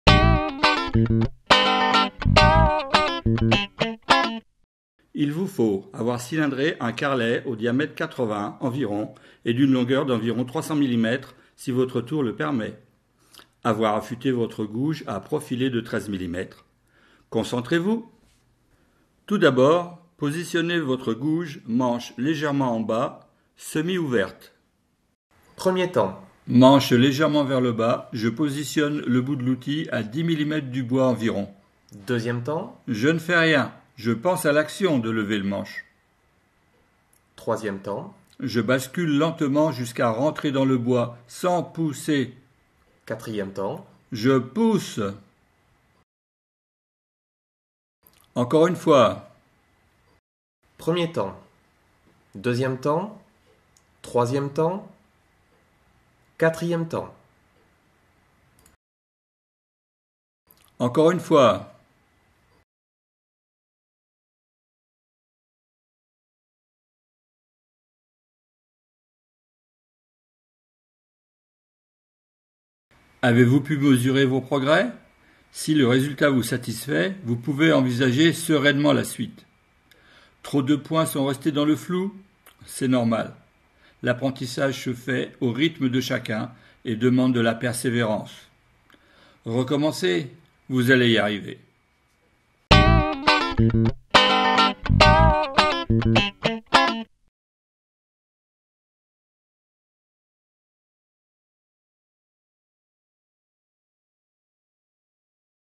Initiation tournage sur bois 4 : Le geste salvateur
Avec Bordet et jetournelebois.com, découvrez comment ne plus accrocher avec une gouge à profiler ou une plane.